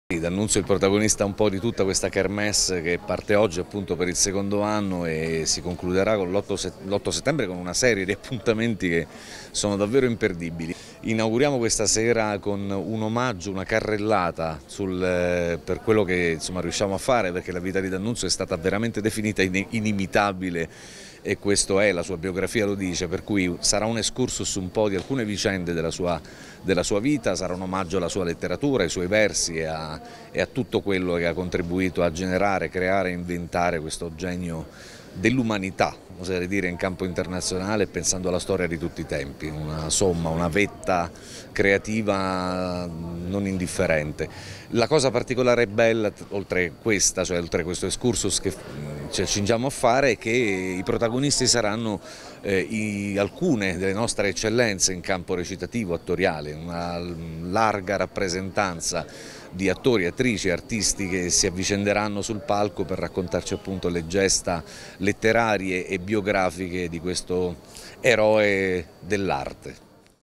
Festa della Rivoluzione, Milo Vallone racconta "Eccomi! il Galà del teatro Dannunziano"
L'attore e regista presenta lo spettacolo che ha dato il via alla seconda edizione della Festa della Rivoluzione all'Aurum di Pescara